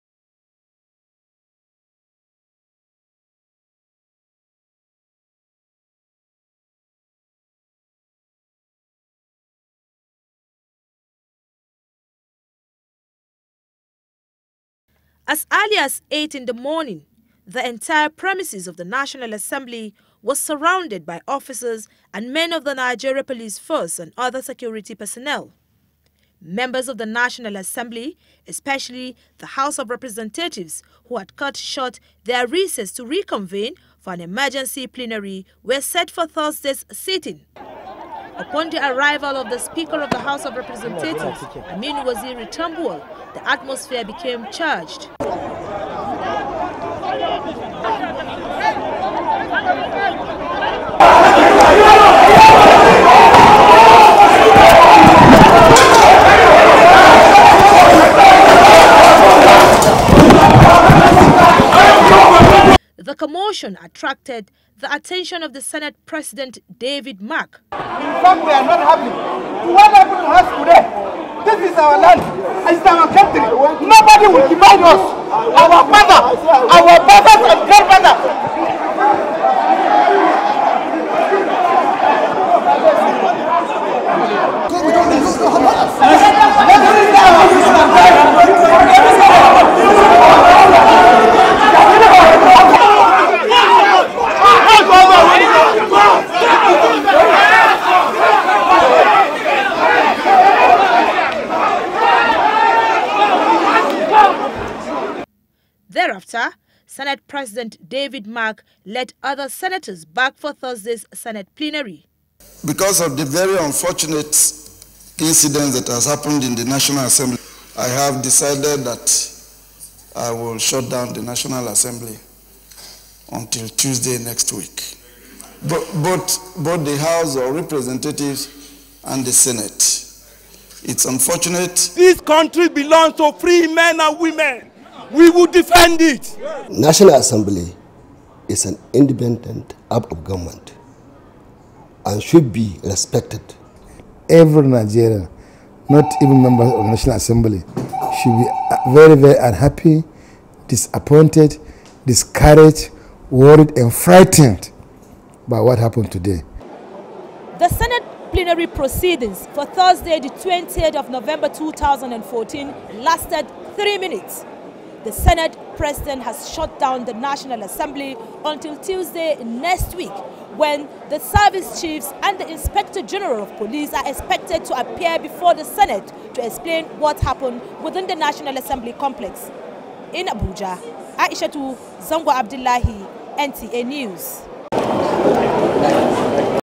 Senate President ordered The Closure Of NASS
Aishatu Zango Abdullahi

The Senate President David Mark has decried the presence of heavy security operatives at the National Assembly obstructing normal operations of the legislature.
Senator David Mark therefore ordered the immediate suspension of Plenary and the closure of the National Assembly till Tuesday the 25th of November 2014.